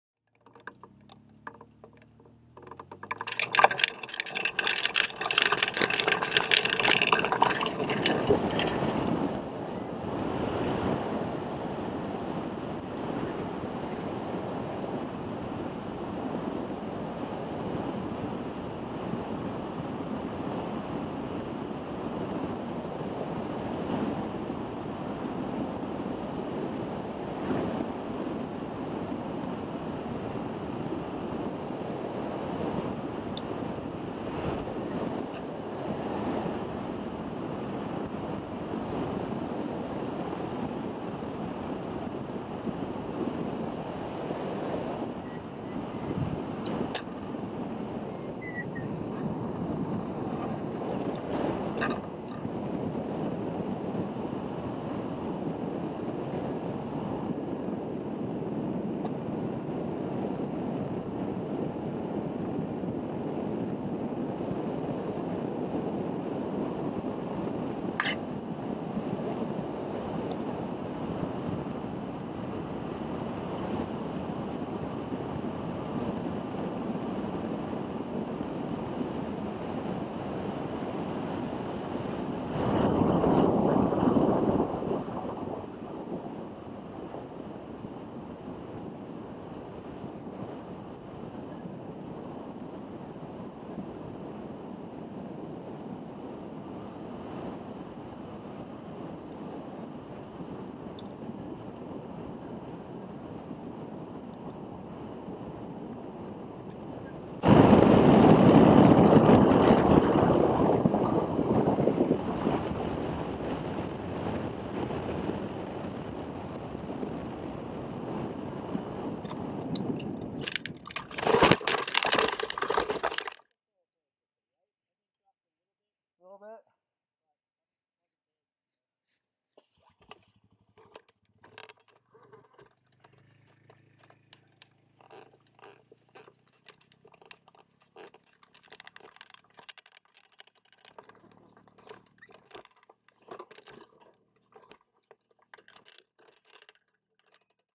Checkout Flight
This video is about hang gliding at Morningside Flight Park.